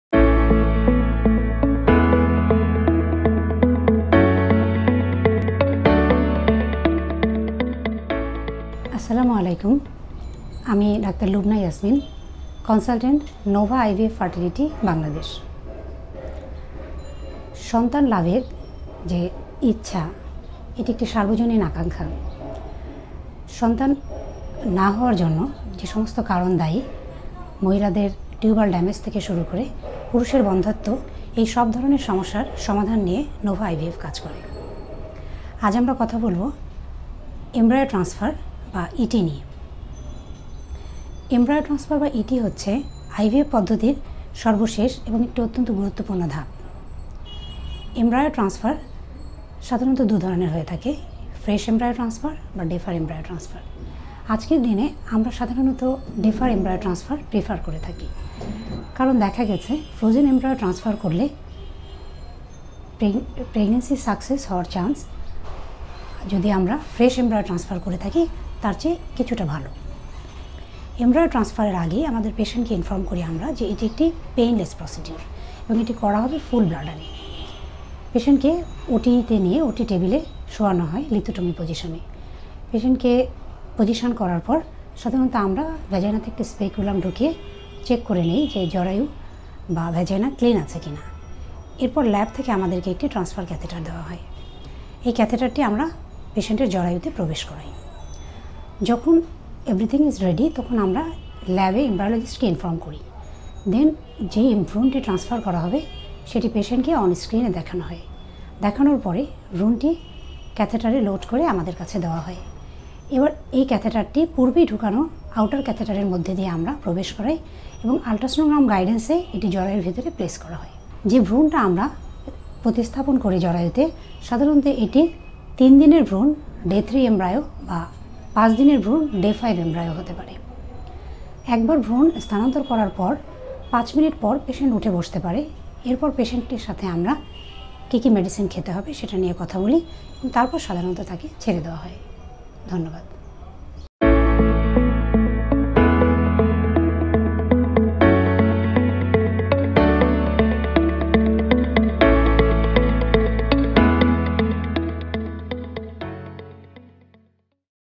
The process of Embryo Transfer explained by Dr Lubna Yasmin, fertility specialist- Nova IVF Dhaka.
Dr Lubna Yasmin, fertility specialist- Nova IVF Fertility Dhaka., talks on Embryo Transfer, the last part of in vitro fertilization (IVF) process. She explains that during IVF, fertility medications are used to stimulate the ovaries into releasing healthy eggs.